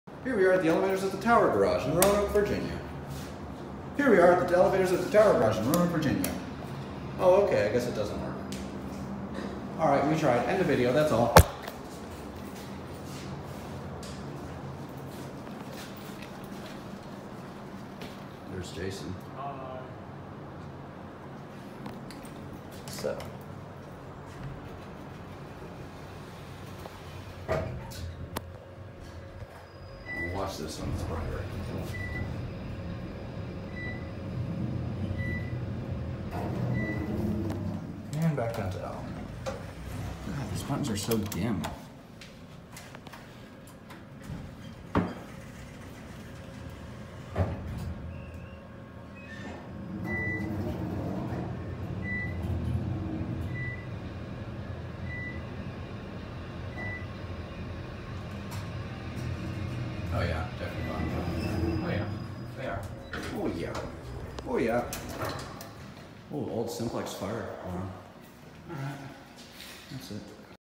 Otis Traction Elevators @ Tower Garage - Roanoke, VA.
TECH SPECS:
Brand: Otis
Model: N/A
Drive: AC Geared Traction
Year Installed: 1990s
Floors Served: 7
Capacity: 3000 lbs
Speed: 350 FPM
Doors: Center Opening

FIXTURES:
Buttons: Epco SSL
Indicator: Otis Series 4 DPI
Lanterns: Otis